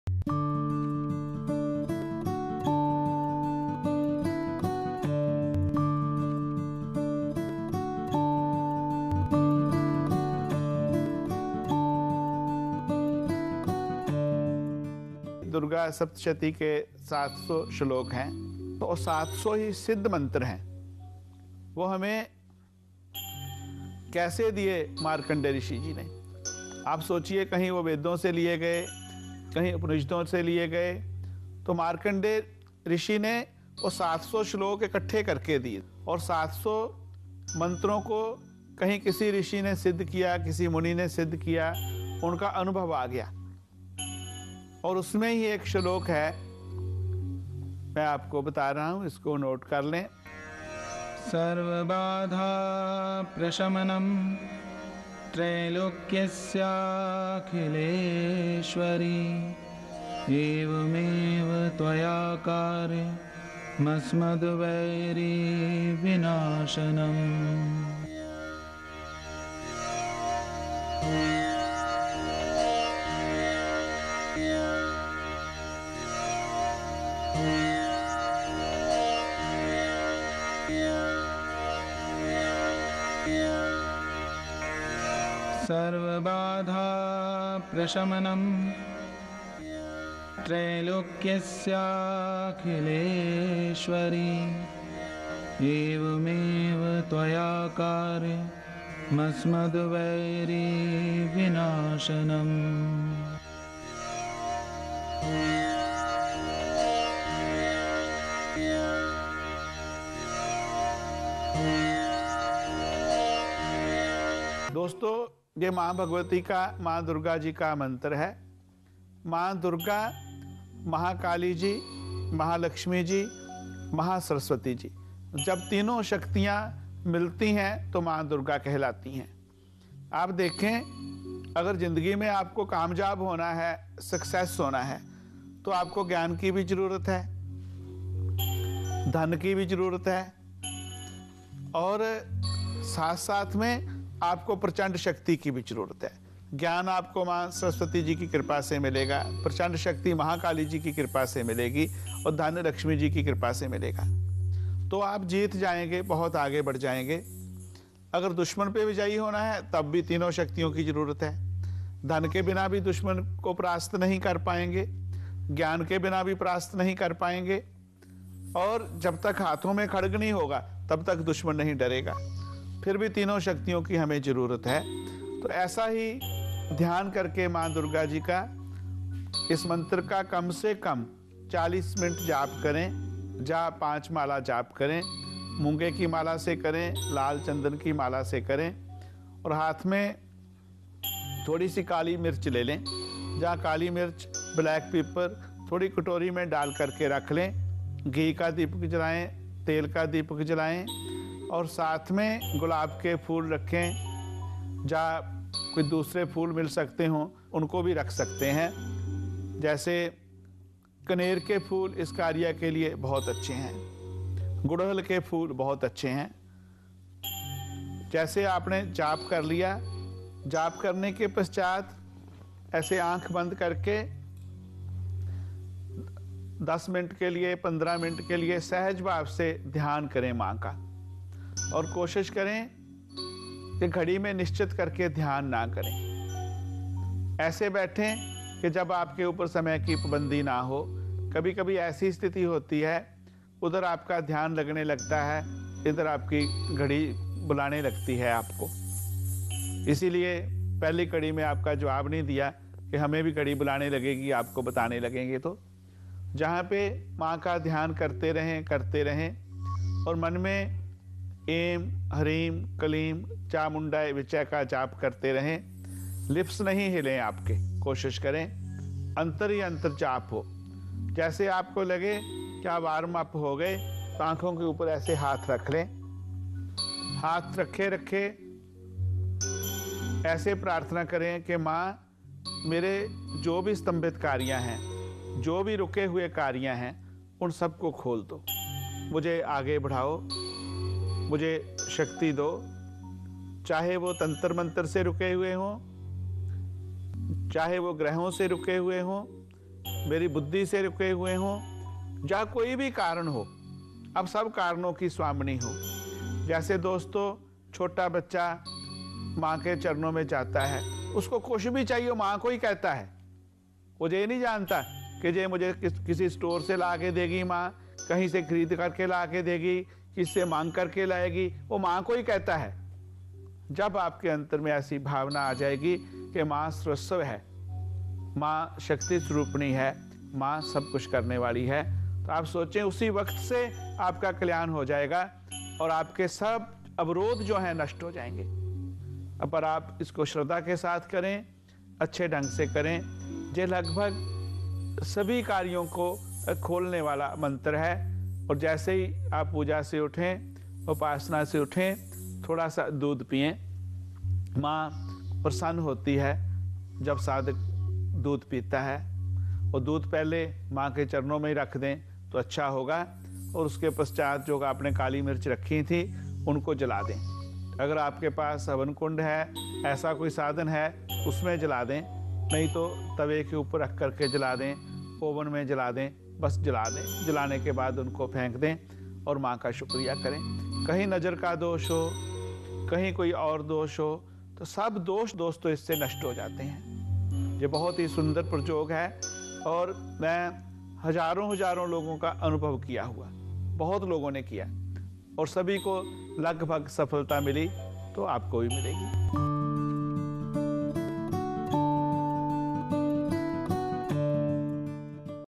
Remedy for a Successful Career
In current times, many people are going through job and career issues. In this video, Swamiji suggests a basic ritual for anyone who is struggling with career obstacles. The ritual needs to be performed for a minimum of forty days for the best results. Try it and share your experiences with us.